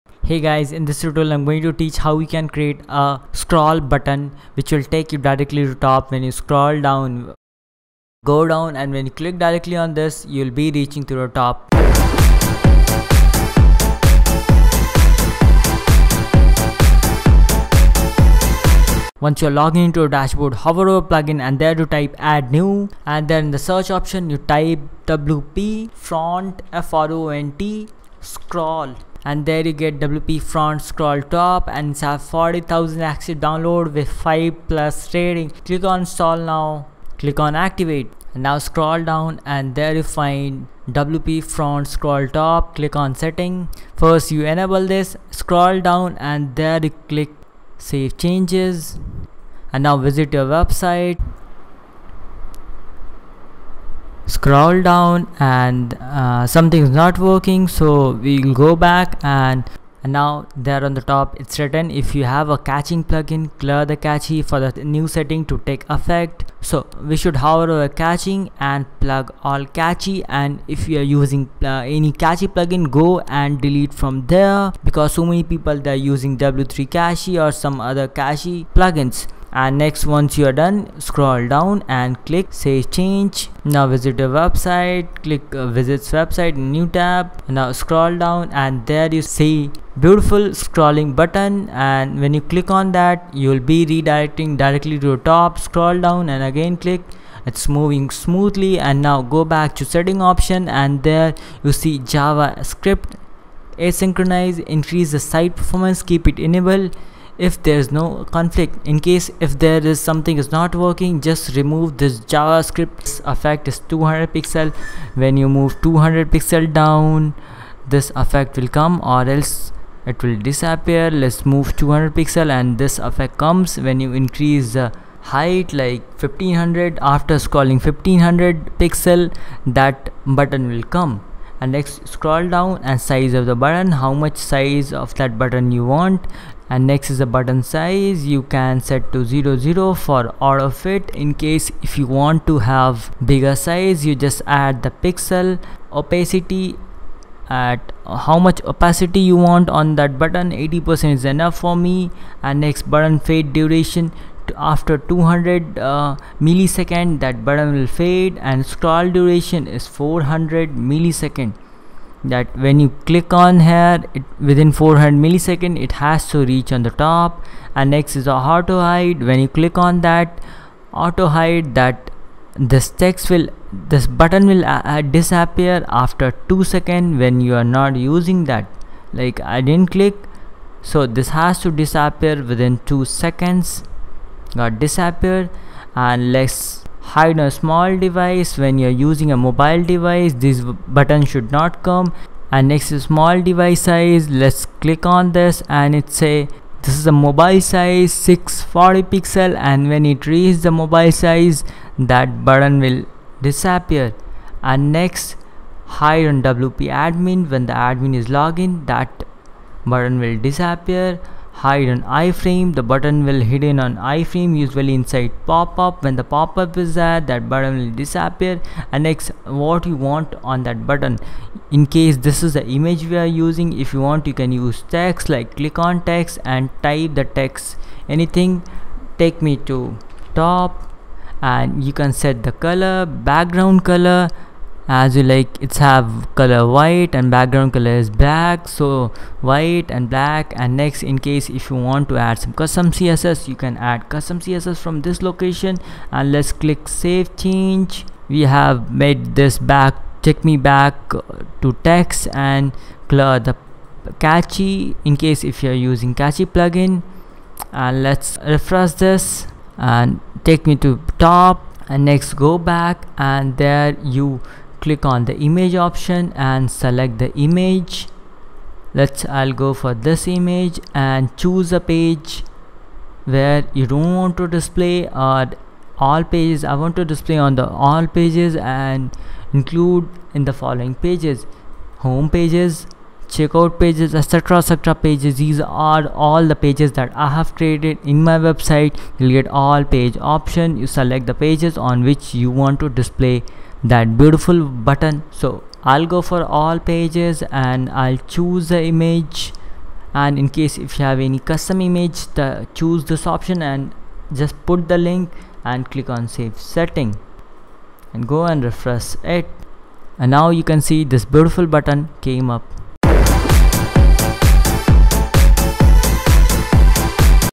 How To Add Scroll To Top In WordPress Website | Wpfront Scroll Top Plugin
Learn Wpfront Scroll Top Plugin | How To make Scroll To Top In One Click To Top Of Your Website. Wpfront Scroll Top Plugin
High Performance Hosting must try